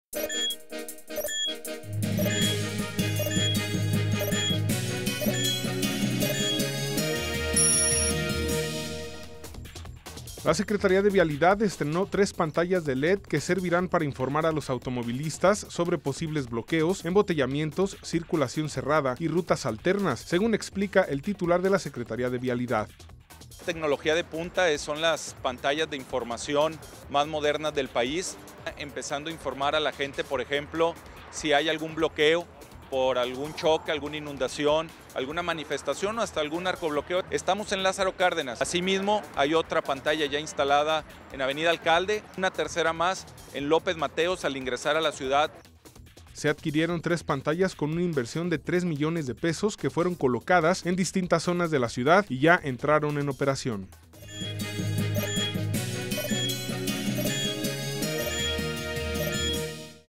Corte 44 - Estrena SVT pantallas informativas en L. Cárdenas, Alcalde y López Mateos - Oct 2 2012
CORTE 44 -- La Secretaría de Vialidad estrenó 3 pantallas de led que servirán para informar a los automovilistas sobre posibles bloqueos, embotellamientos, circulación cerrada y rutas alternas. Se adquirieron 3 pantallas con una inversión de 3 millones de pesos, que fueron colocadas en distintas zonas de la ciudad y ya entraron en operación.